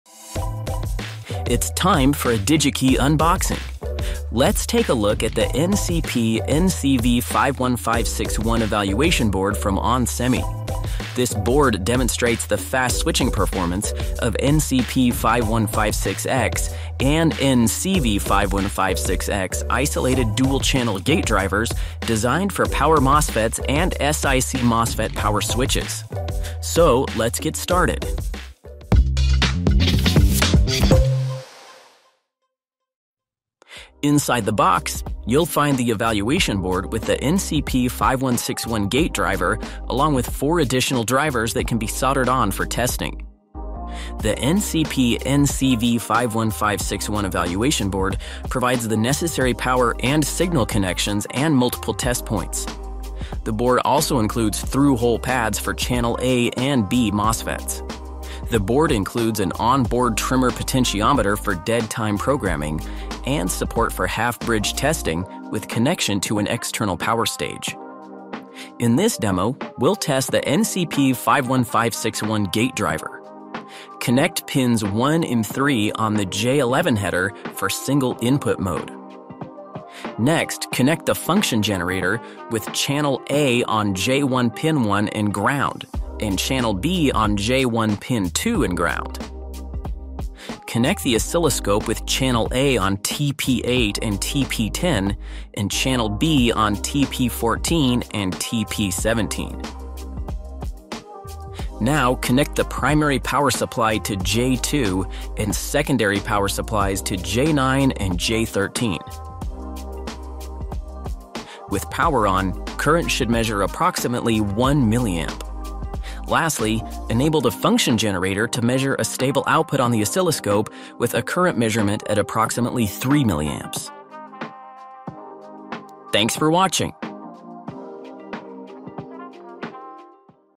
onsemi NCP-NCV51561 Gate Driver - Unboxing | DigiKey
Inside the box you’ll find the evaluation board with the NCP5161 gate driver along with 4 additional drivers that can be soldered on for testing.

The NCP NCV51561 evaluation board provides the necessary power and signal connections and multiple test points. The board also includes through hole pads for channel A and B MOSFETs.
The board includes an onboard trimmer potentiometer for dead time programming and support for half bridge testing with connection to an external power stage.

In this demo we’ll test the NCP51561 gate driver. Connect pins 1 and 3 on the J11 header for single input mode. Next, connect the function generator with Channel A on J1-pin-1 and ground, and Channel B on J1-pin-2 and ground. Connect the oscilloscope with channel A on TP8 and TP10, and channel B on TP14 and TP17.

Now connect the primary power supply to J2, and secondary power supplies to J9 and J13. With power on, current should measure approximately 1 milliamp.

Lastly, enable the function generator to measure a stable output on the oscilloscope with a current measurement at approximately 3 milliamps.